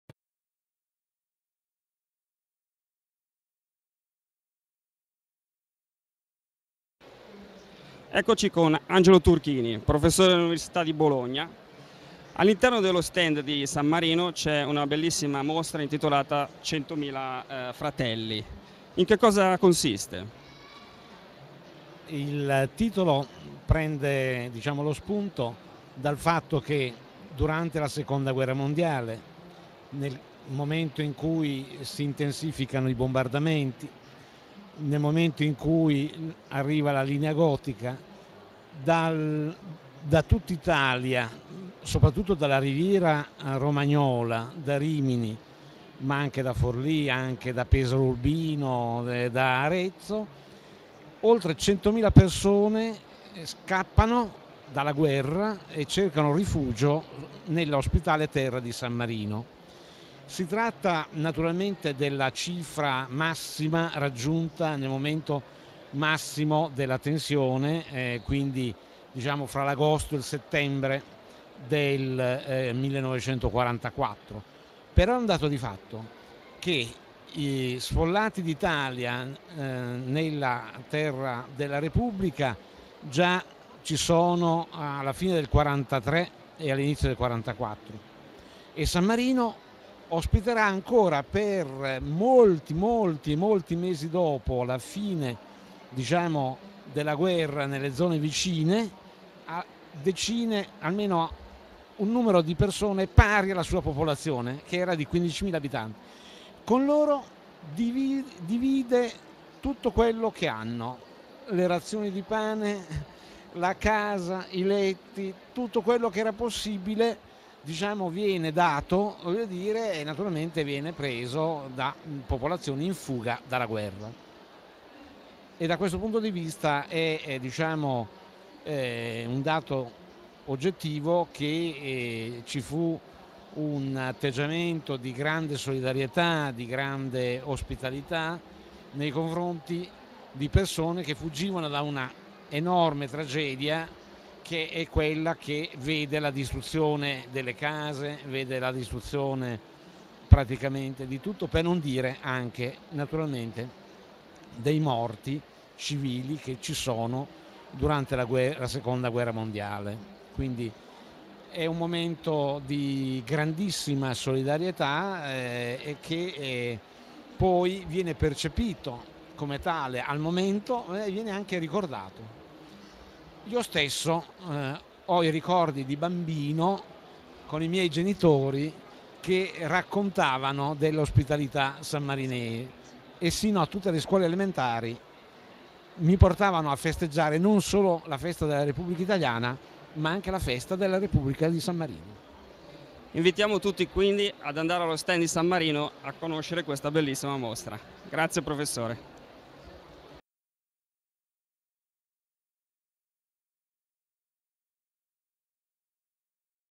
Angelo Turchini, professore ordinario all'Università di Bologna
Il prof. Turchini spiega la mostra "Centomila Fratelli" allo stand della rep. di San Marino: durante la Seconda Guerra Mondiale sul monte Titano verranno accolti centomila italiani.